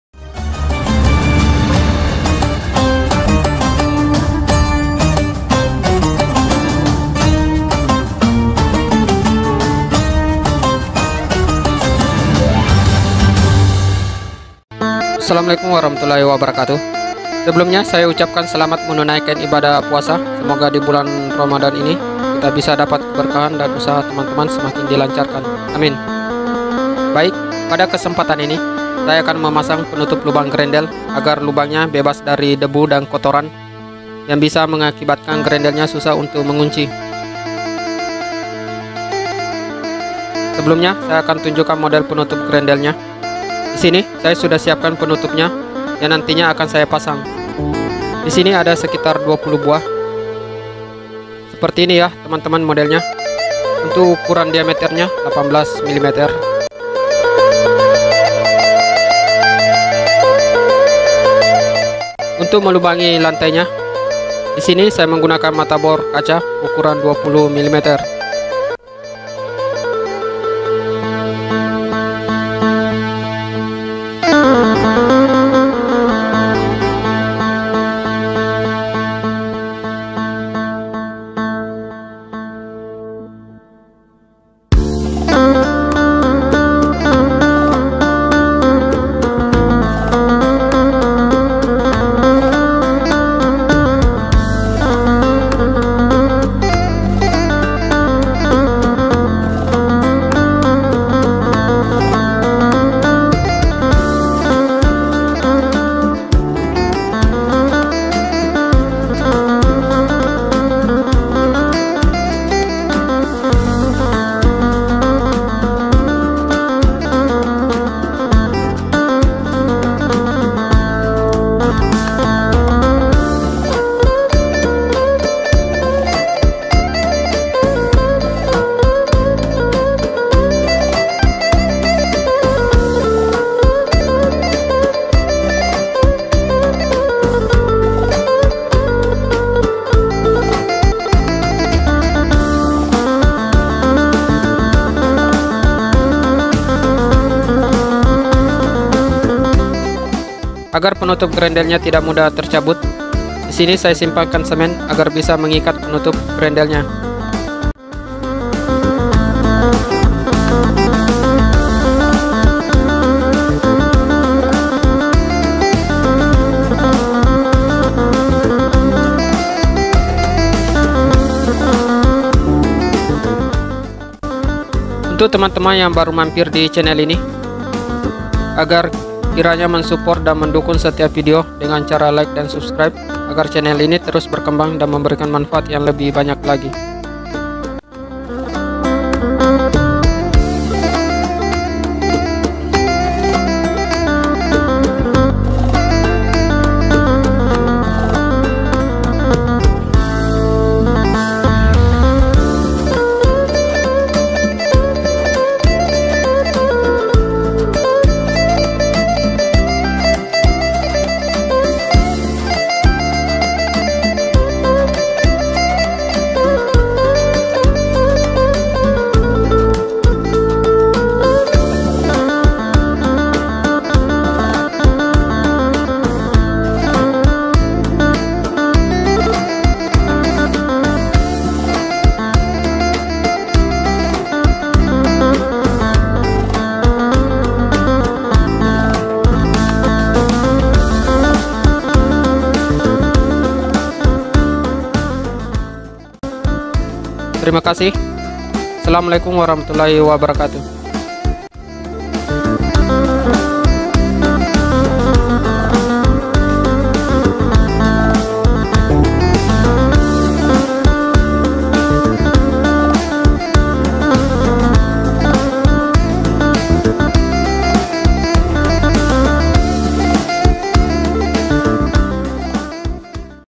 ⭕⭕⭕CARA PASANG PENUTUP LUBANG GRENDEL💯
pada kesempatan ini saya akan memasang penutup lubang grendel agar lubangnya bebas dari debu dan kotoran yang bisa mengakibatkan grendelnya susah untuk mengunci.
Penutup lubang grendel ini nantinya dipasang dilantai.